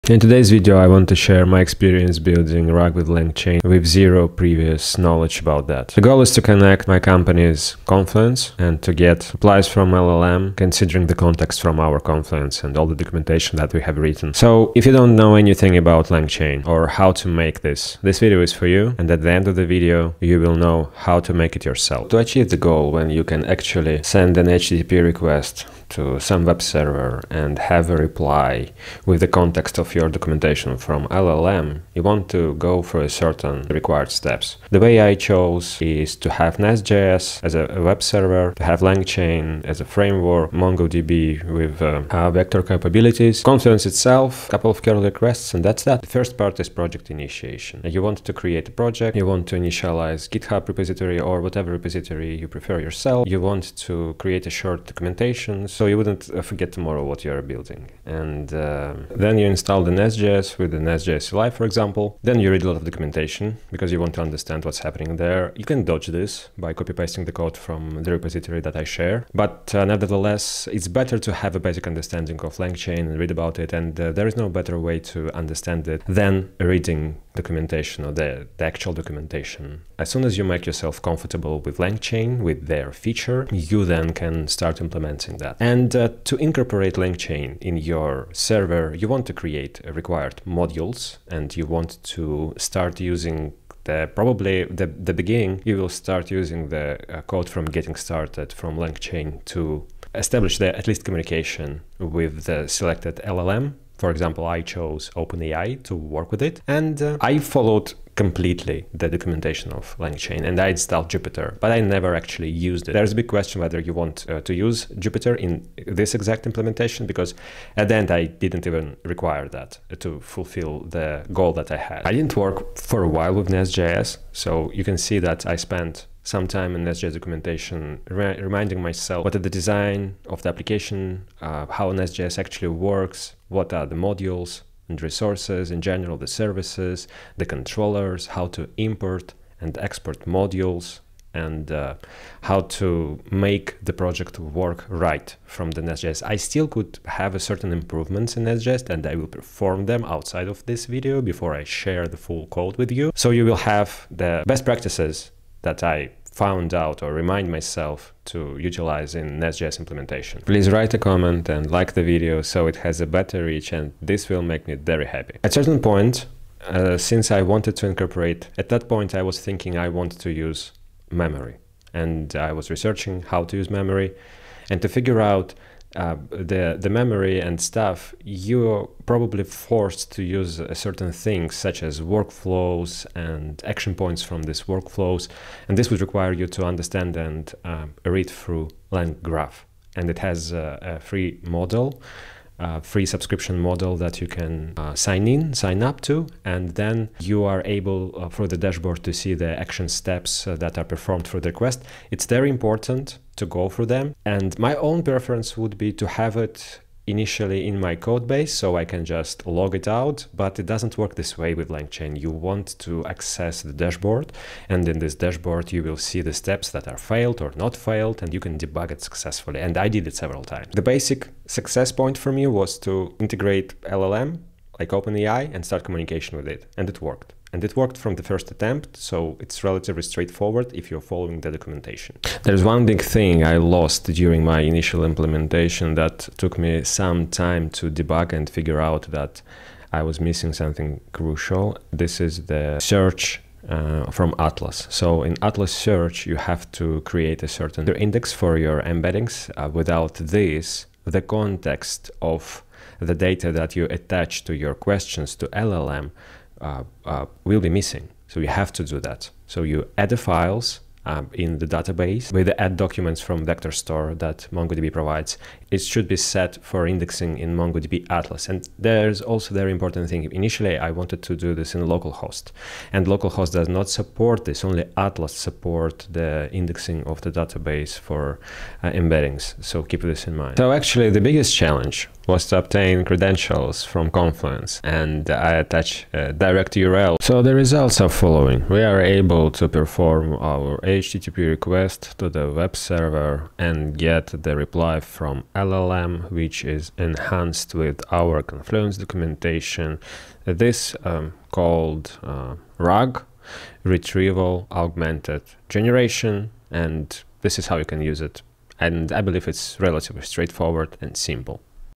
Build RAG with LangChain, NestJS, and Confluence: From Zero to Hero in 5 minutes
In this video, I am building a complete Retrieval Augmented Generation (RAG) system that integrates your Confluence documentation with LLMs—without any prior knowledge of LangChain.

If you want to get the full codebase from this video - join